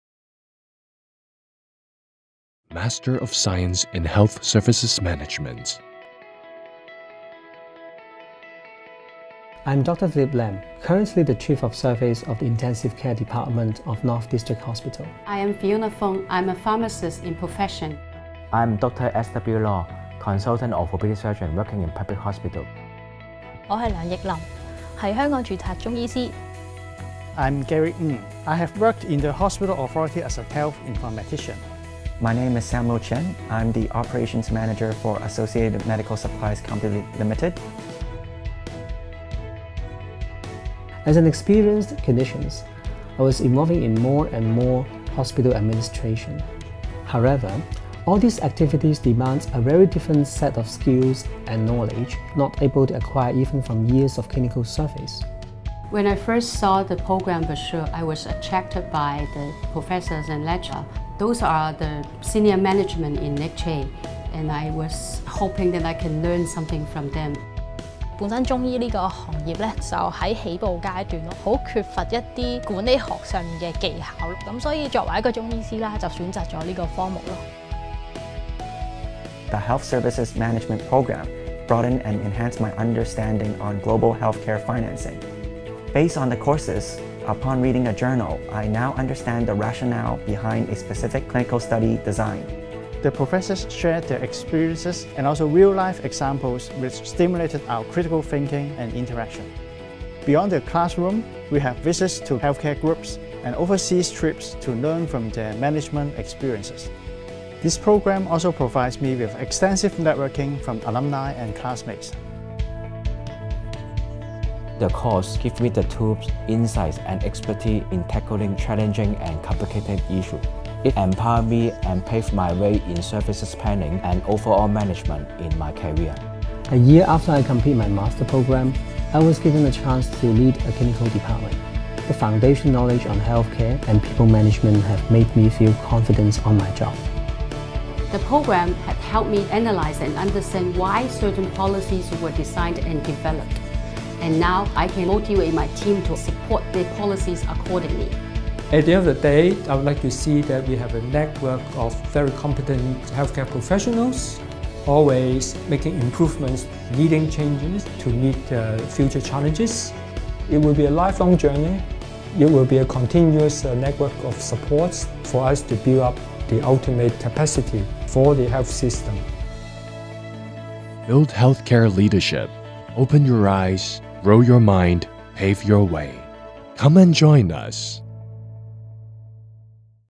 中大醫療管理學理學碩士課程Master of Science in Health Services Management@CUHK
醫療管理學理學碩士課程 (兩年制兼讀課程)
Master of Science in Health Services Management (HSM). 2-year part-time study.

BUILD HEALTHCARE LEADERSHIP- OPEN your EYES,GROW your MIND, PAVE your WAY
Master of Science in Health Services Management programme is a multidisciplinary field of inquiry and practice concerned with the delivery, quality and costs of healthcare for individuals and populations.

Application deadline for 2021-22 intake: 1 May 2020 (full-time study). 31 May 2021 (part-time study).
Information information and taste lecture.

(Video first released in Apr. 2017)